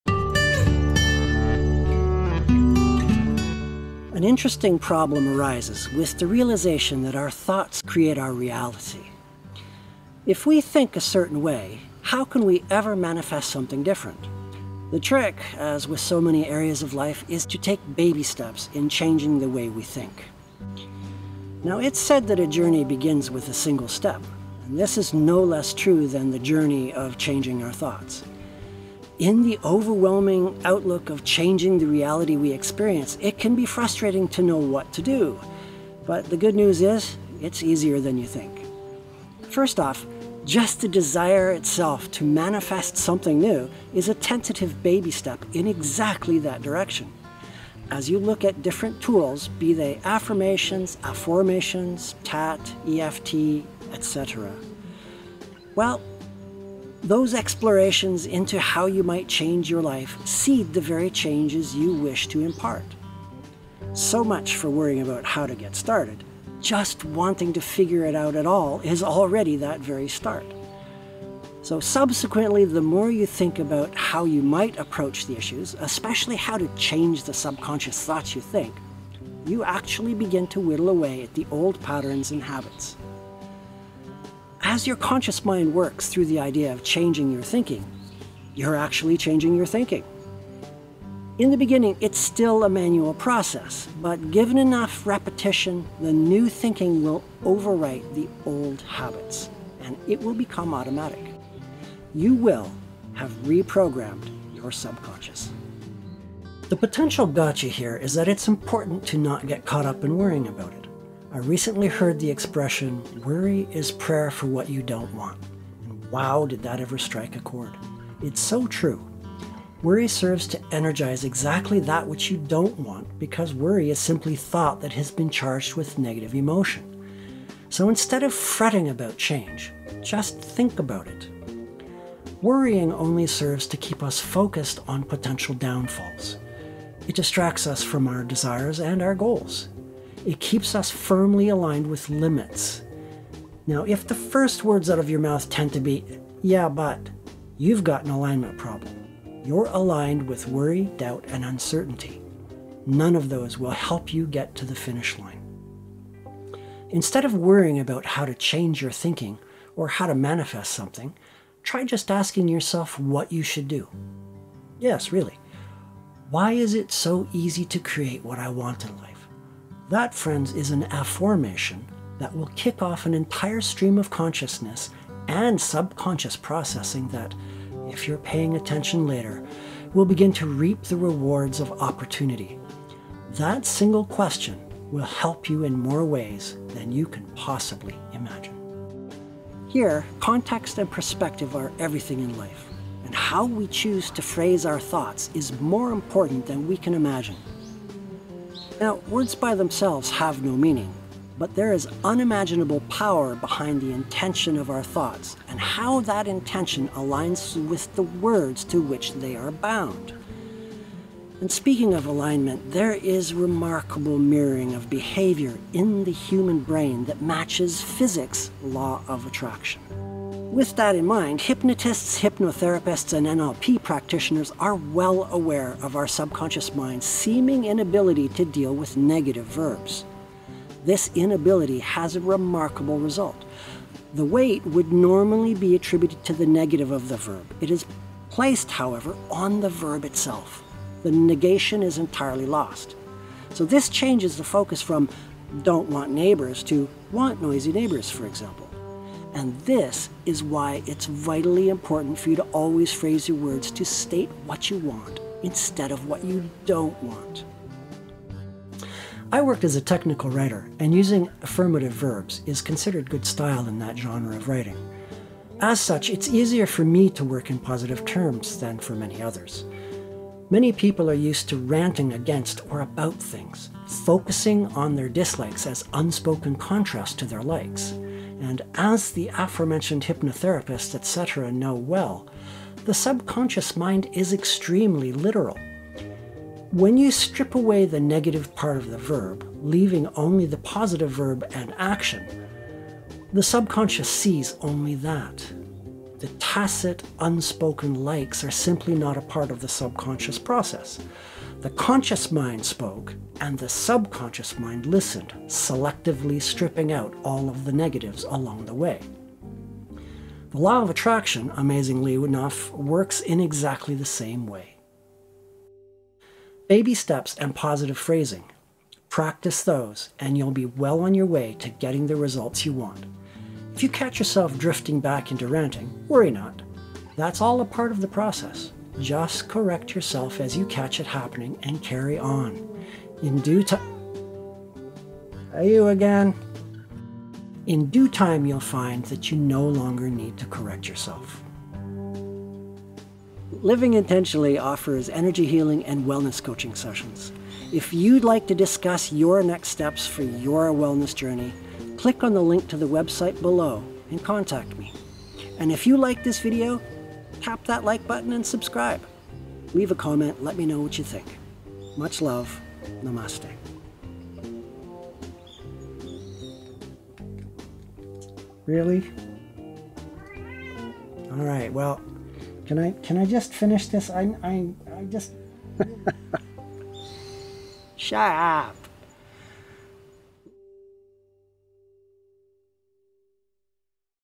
Clear Limiting Beliefs
How we frame our thoughts has a huge impact on how we experience life. As much as possible, always state what you WANT instead of what you don't want. The subconscious mind -- and Law of Attraction! -- strips away negatives such that "I don't want to be sick" becomes "I want to be sick". As the genie says, "Your wish is my command!" Turn it around to say what you want: "I want to be healthy!" Frame everything in the affirmative voice and your LIFE WILL CHANGE!